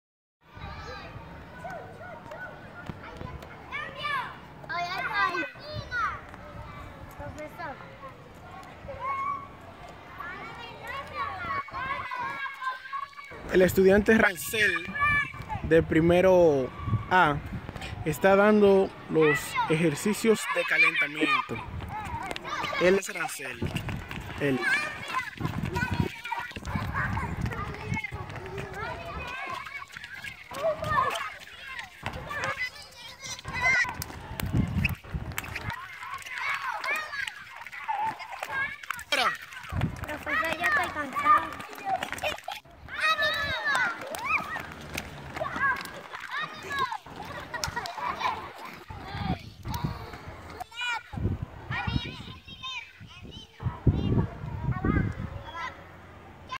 Calentamiento General en educación física
Un estudiante de primer grado de nivel primario muestra a sus compañeros a como hacer los ejercicios